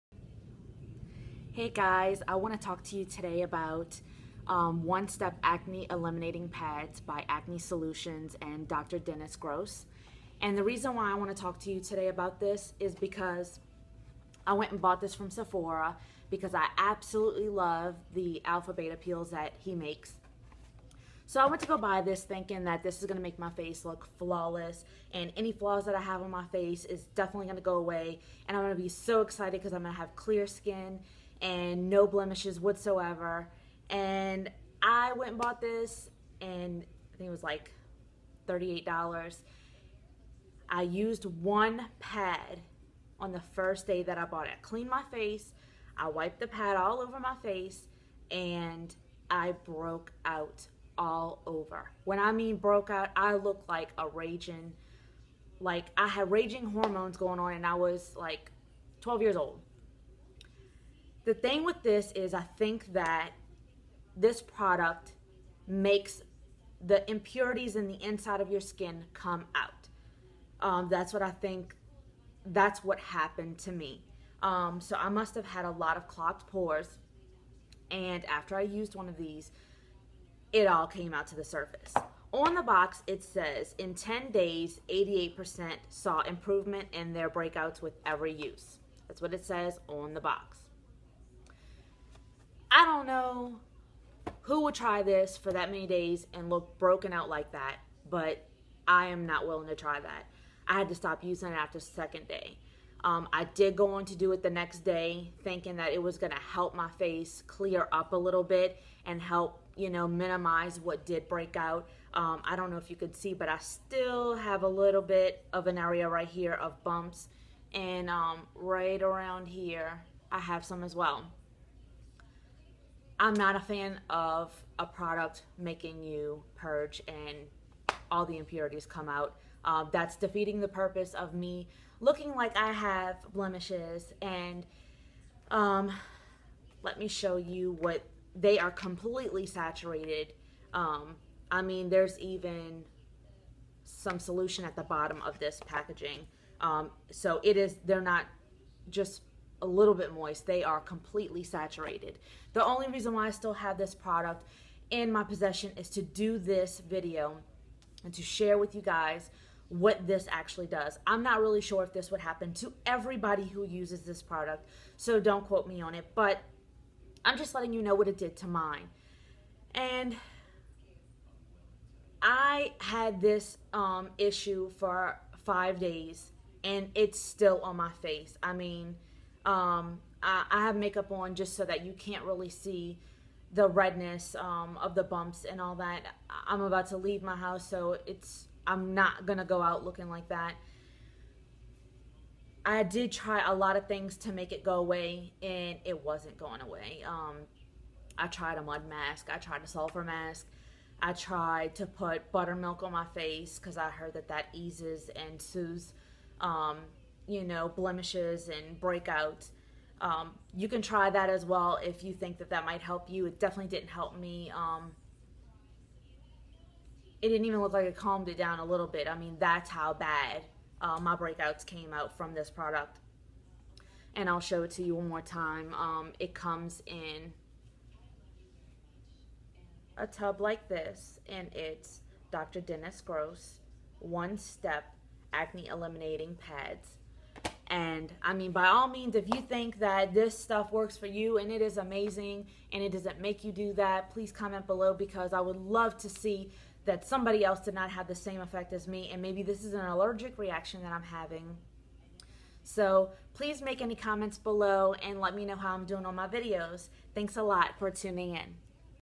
Dr.dennis Gross one step acne pads review
Review on one step acne pads by dr.Dennis Gross ... I hate how these pads broke me out and made my face red and sore